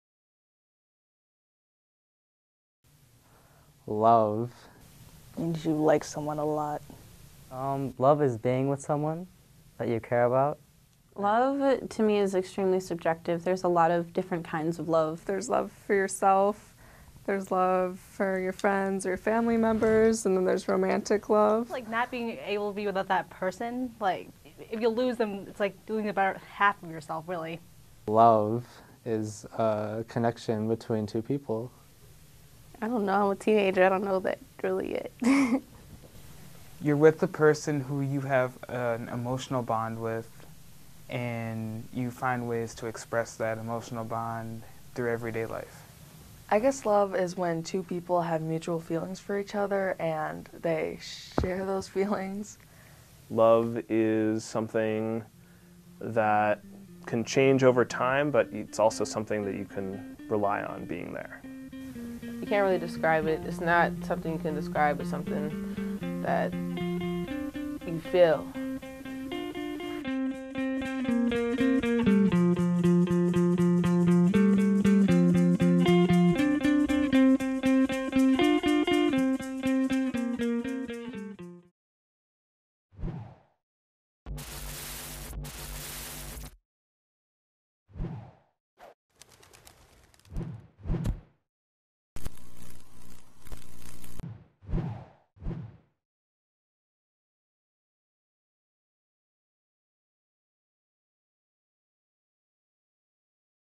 What does love mean?
The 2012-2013 crew talks about what it means to love someone in this segment from Episode 1202 on relationships.